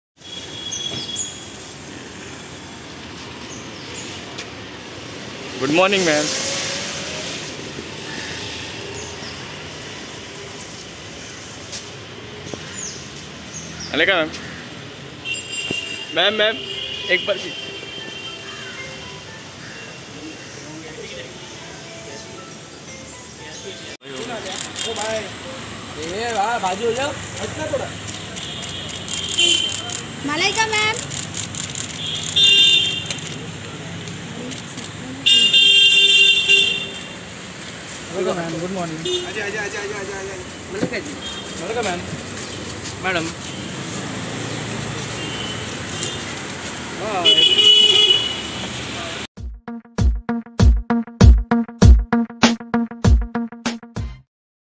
FitnessFriday: Malaika Arora’s fitness routine is absolutely noteworthy!
Malaika is one of the tinsel town’s fittest people and knows how to dress with grace, confidence, and flair. Being a fitness freak, she often shares her views on intense workouts. Recently, the diva was spotted outside her own Yoga centre post-workout. The diva looked flawless as always. Malaika Arora was last seen as one of the judges in the dance reality show India's Best Dancer, alongside choreographers Terence Lewis and Geeta Kapur.